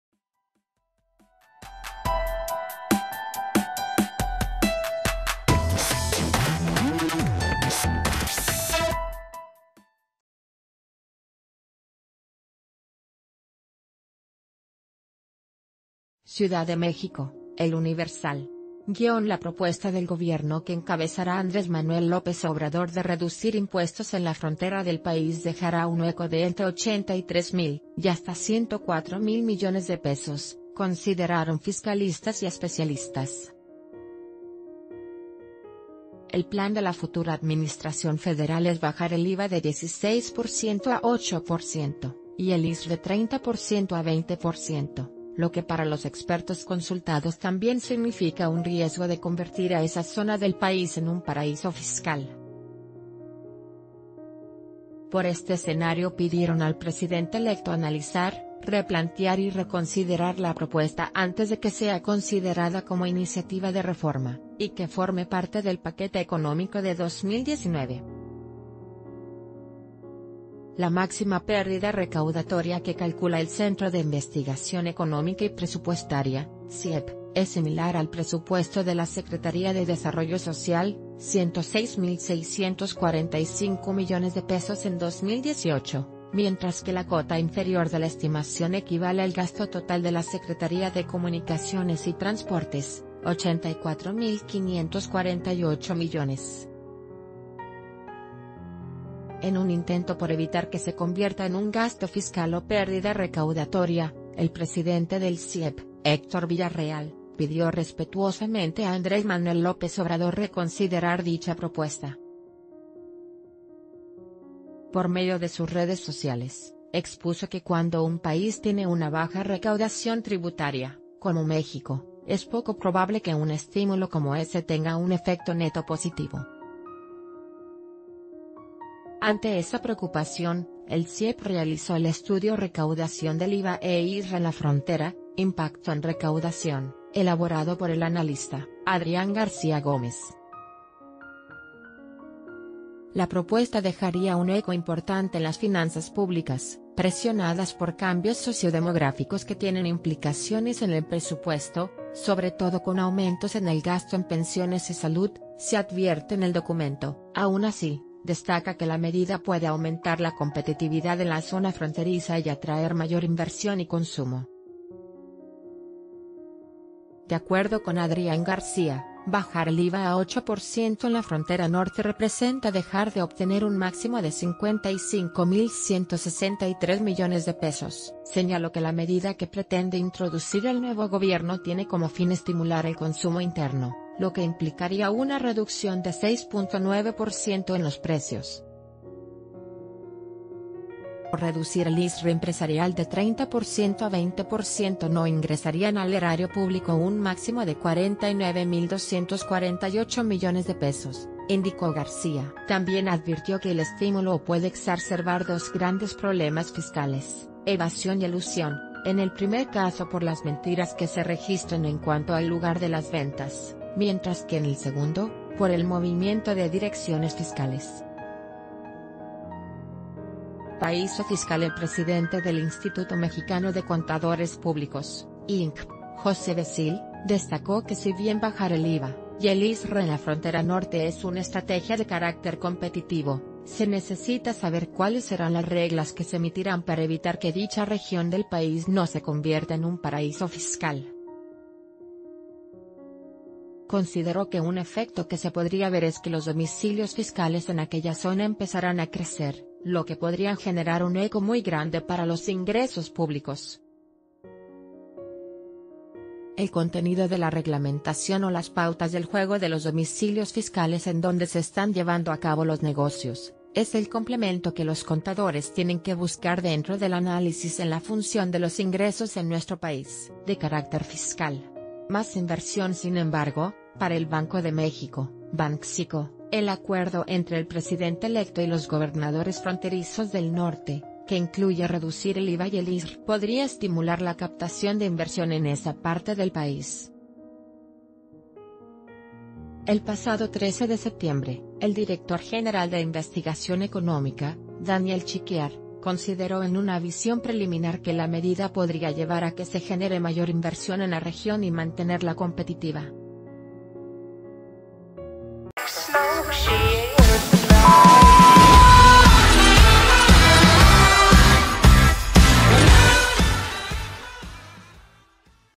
Reducir IVA e ISR abrirá boquete de 104 mil mdp - El Diario de Yucatán
CIUDAD DE MÉXICO (El Universal).- La propuesta del gobierno que encabezará Andrés Manuel López Obrador de reducir impuestos en la frontera del país dejará un hueco de entre 83 mil y hasta 104 mil millones de pesos, consideraron fiscalistas y especialistas. El plan de la futura administración federal...